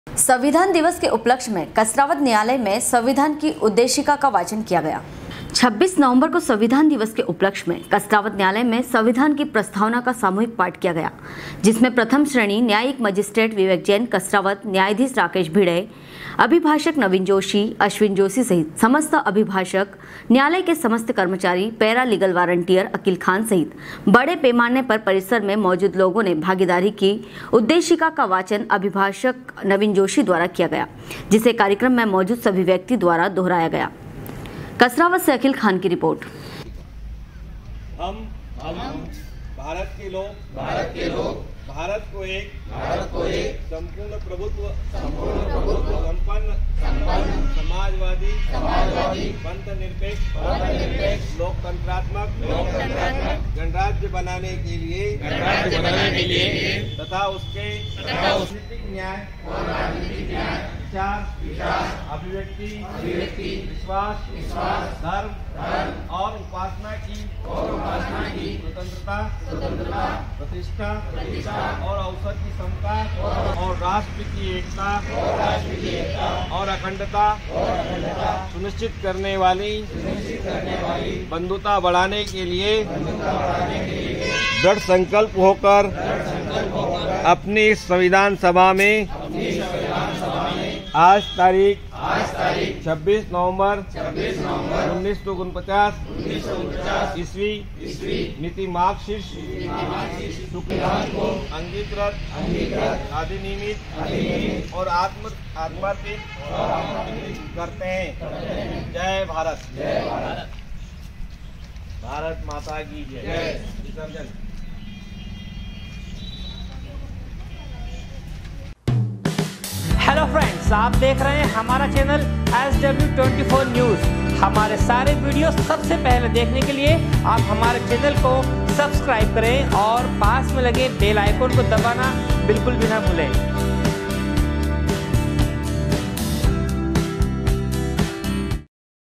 SW 24 News Madhyapradesh
kasrawad सविधान दिवस के उपलक्ष में कसरावद न्यायालय में संविधान की उद्देशिका का वाचन किया गया